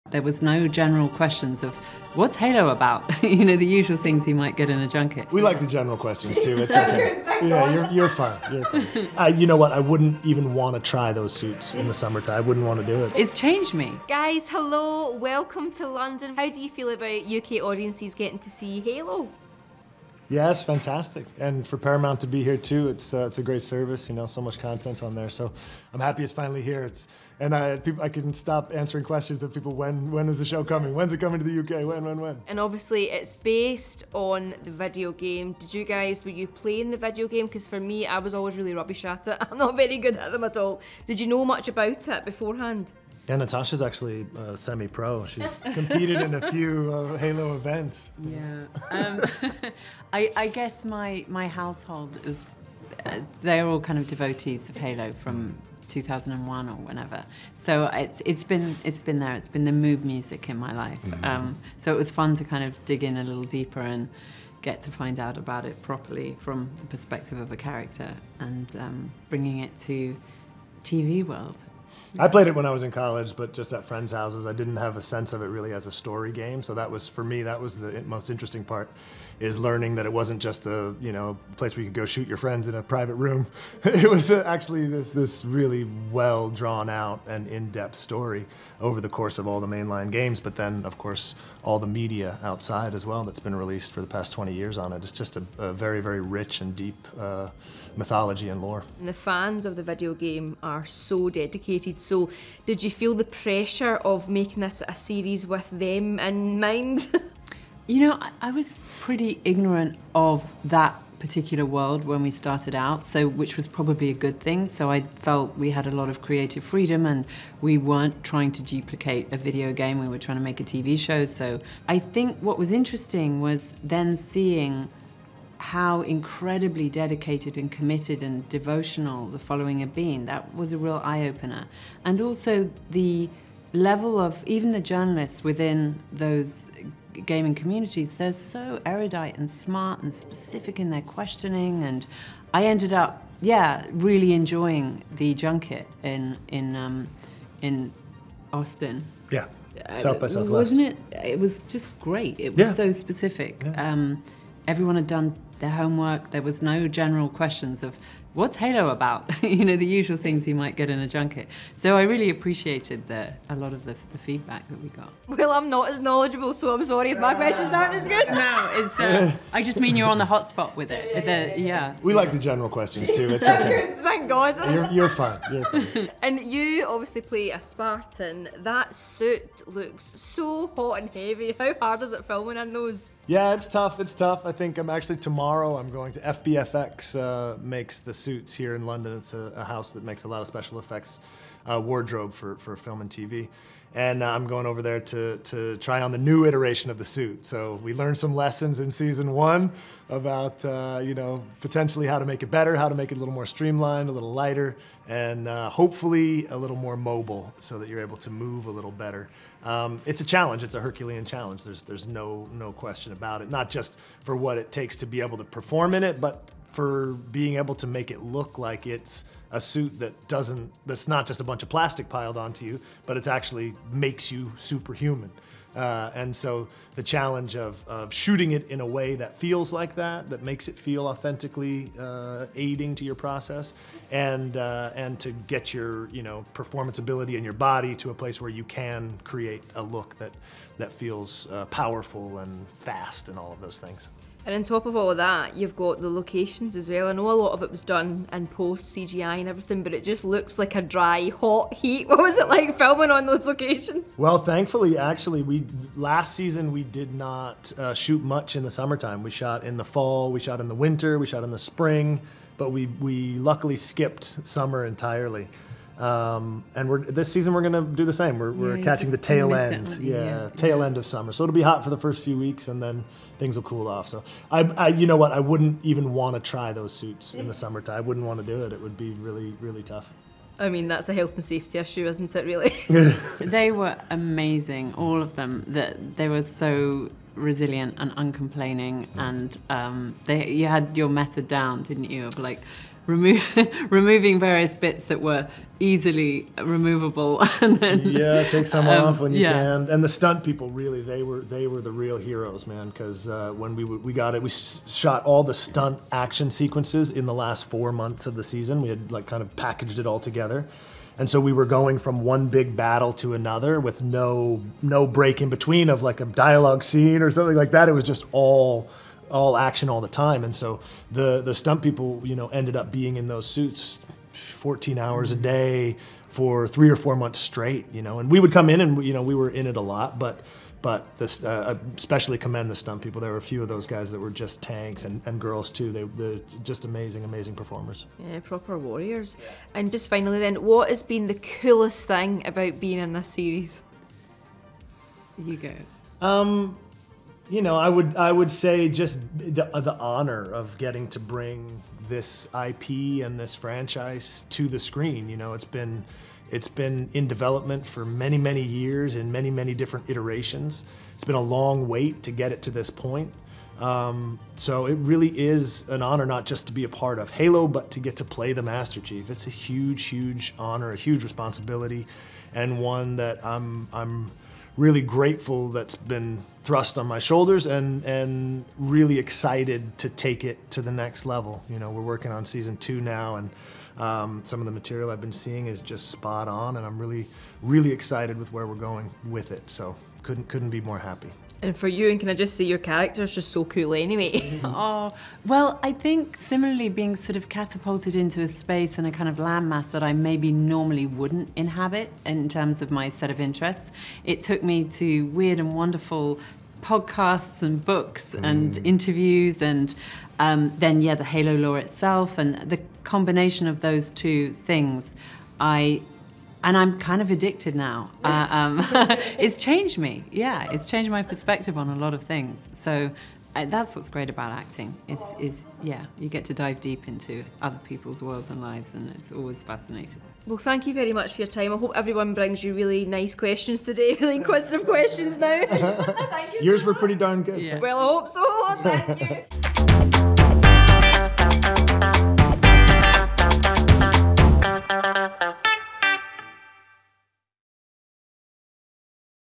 Natascha McElhone & Pablo Schreiber on HALO Landing in UK!
To celebrate Paramount+ launching in the UK, our Jodie McCallum sat down with the stars of HALO! Yes, the popular video game has been turned into a TV series.

Dramatizing an epic 26th-century conflict between humanity and an alien threat known as the Covenant, Halo the series will weave deeply drawn personal stories with action, adventure and a richly imagined vision of the future.

The series stars Pablo Schreiber (Master Chief, John-117), Natascha McElhone (Dr. Halsey), Jen Taylor (Cortana), Bokeem Woodbine (Soren-066) and more.

Will you be watching the series?

Report by Jodie Mccallum.